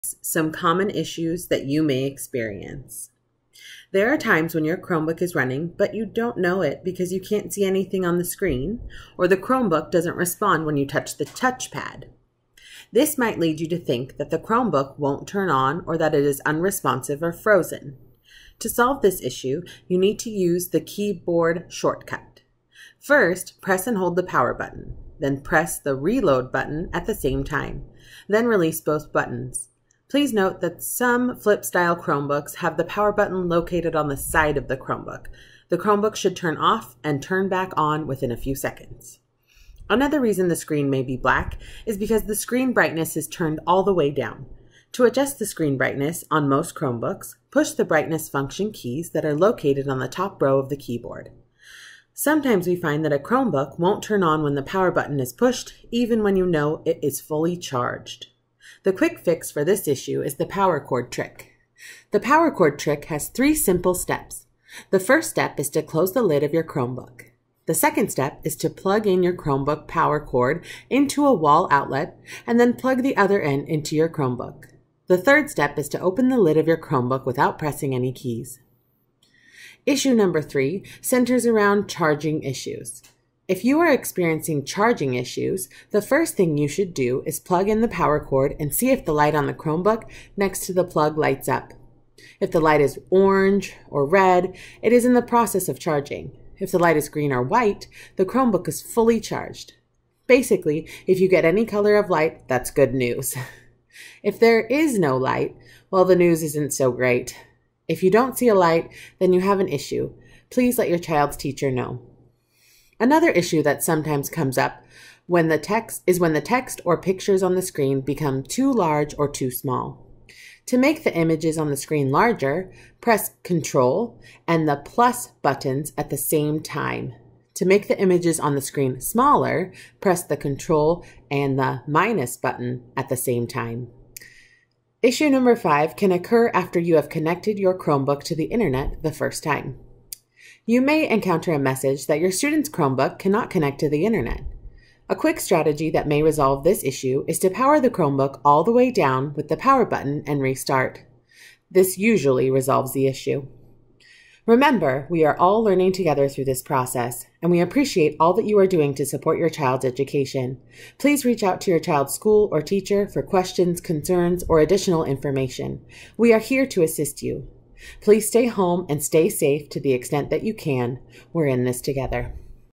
Troubleshooting Chromebooks
This video will walk you through 5  Quick and Easy Chromebook Troubleshooting Tips to Fix some common issues that you may experience.